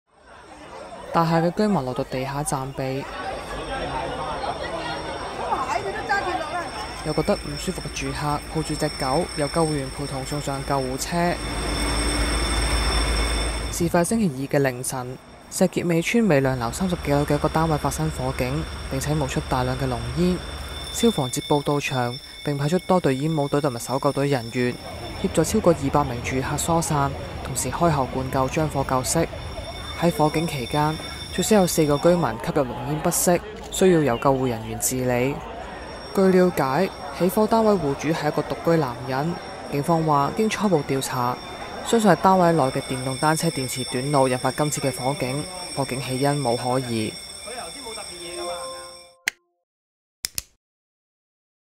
石硤尾邨單位起火4人不適 超過200名居民漏夜疏散｜01新聞｜消防｜電動單車｜短路｜疏散｜救護
石硤尾邨單位起火4人不適　超過200名居民漏夜疏散
石硤尾發生火警。凌晨零時約45分，據報石硤尾邨美亮樓30幾樓一個單位發生火警，多名居民報案。消防接報趕至，派出多隊煙帽隊及搜疏隊人員，協助超過200名住客疏散到地面安全位置暫避；同時開喉灌救。火警期間，有最少4名居民吸入濃煙不適，須由救護員治理，其中一名女子送上救護車時手抱愛犬。據了解，起火單位戶主為一名獨居男子，當時正為其電動單車充電。消防正調查起火原因。